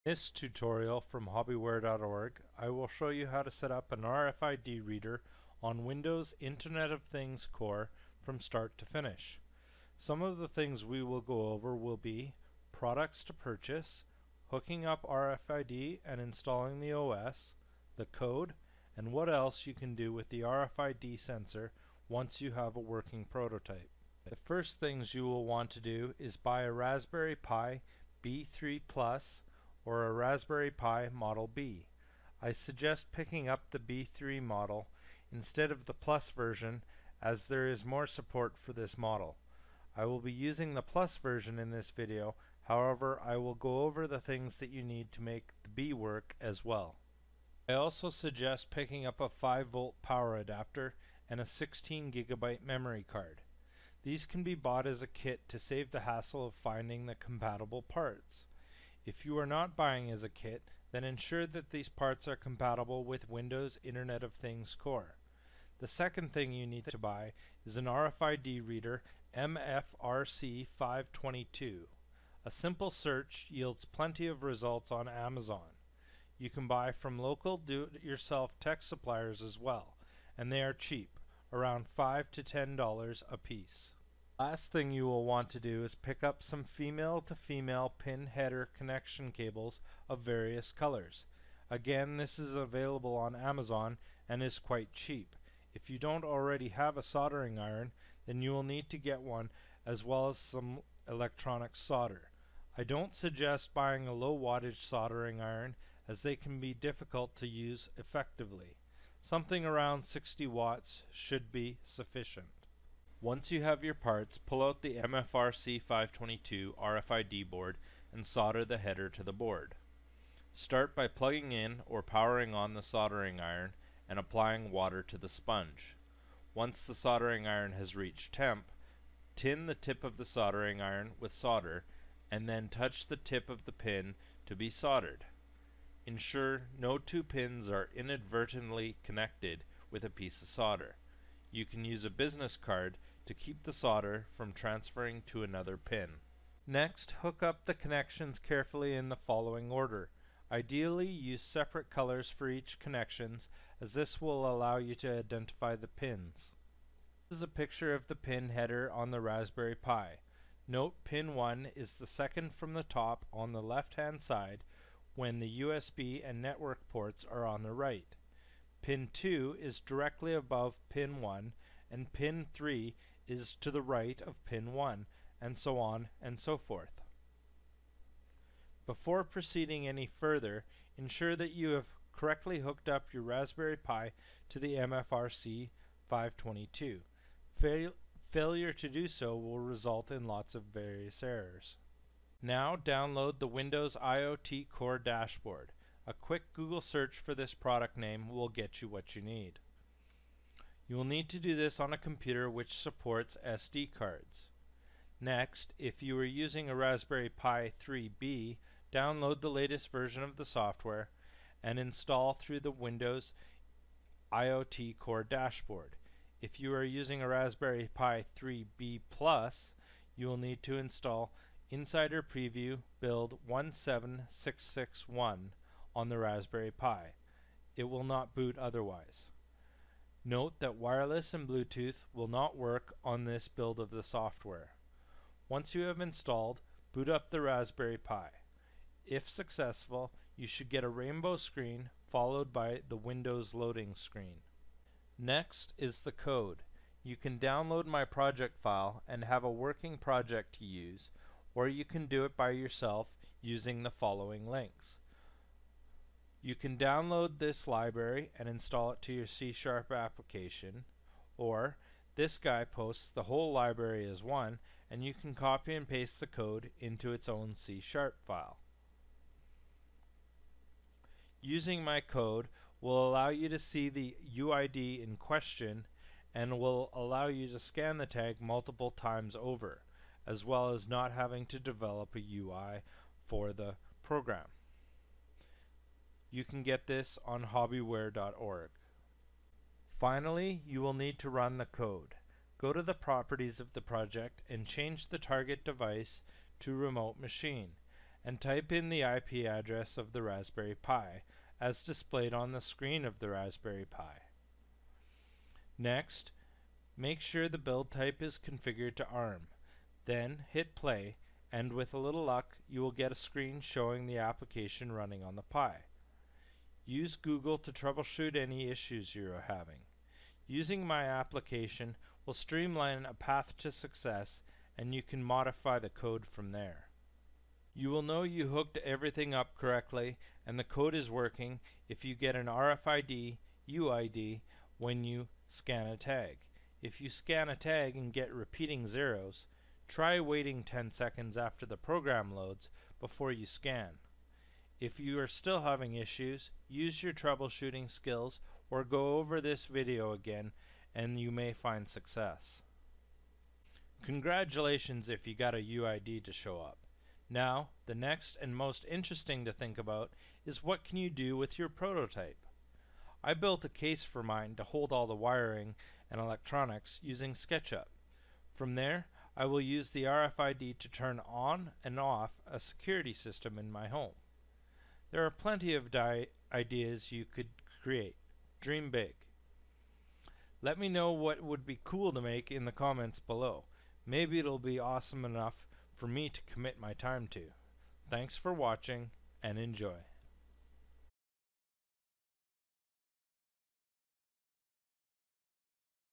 Setting Up Raspberry Pi RFID
Start to Finish getting RFID working on a Raspberry Pi using Windows IOT Core.